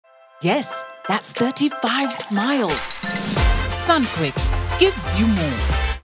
One bottle of Sunquick makes 35 glasses.
At Sunquick, we believe life should be filled with joy that should be shared – whether you’re at home, on the go, together with friends and family – on special and everyday occasions.

Enjoying a glass of Sunquick is as easy as .. ...
1) Shake the bottle like you mean it
2) Pour & mix with water

3) Stir & enjoy!

Grab your Sunquick bottle today for MORE great taste!